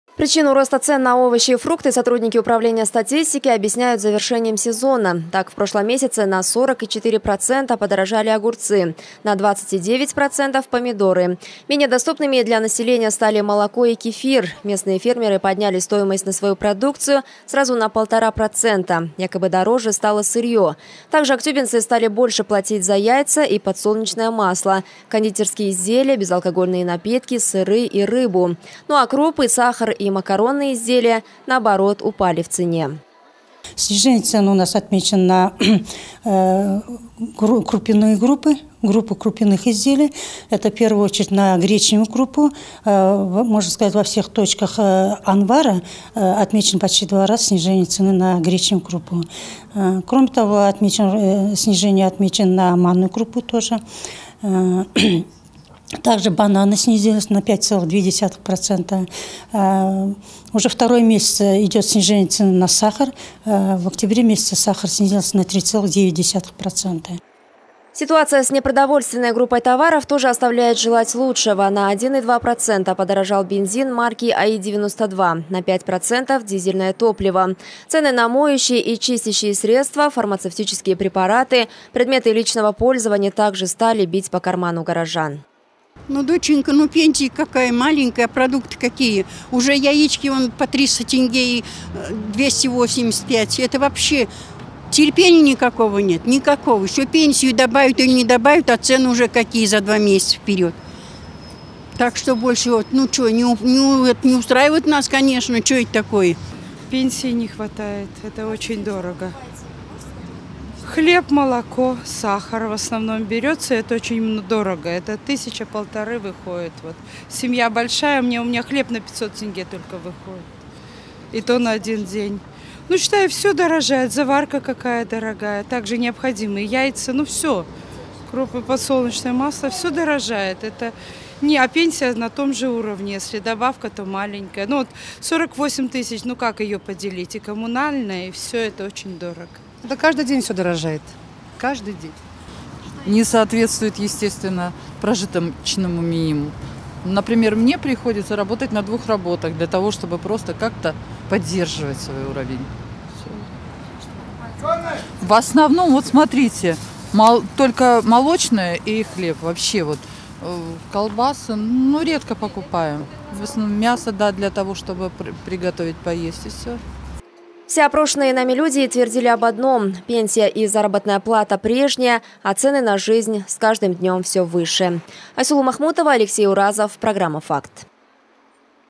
Цены на продовольственные товары в Актобе продолжают расти
Цены на продовольственные товары в Актобе продолжают расти. В частности, в октябре подорожали такие виды продуктов питания, как сыры, рыба, овощи свежие и консервированные, яйца и молочные изделия. Кроме того, на полтора процента повысилась стоимость кондитерских изделий и безалкогольных напитков.
Причину роста цен на овощи и фрукты сотрудники управления статистики объясняют завершением сезона. Так, в прошлом месяце на 40,4% подорожали огурцы, на 20,9% - помидоры. Менее доступными для населения стали молоко и кефир. Местные фермеры подняли стоимость на свою продукцию сразу на полтора процента, якобы дороже стало сырье. Также актюбинцы стали больше платить за яйца и подсолнечное масло, кондитерские изделия, безалкогольные напитки, сыры и рыбу. Ну а крупы, сахар и макаронные изделия, наоборот упали в цене.
Ситуация с непродовольственной группой товаров тоже оставляет желать лучшего. На 1,2% подорожал бензин марки АИ-92, на 5% - дизельное топливо. Цены на моющие и чистящие средства, фармацевтические препараты, предметы личного пользования также стали бить по карману горожан.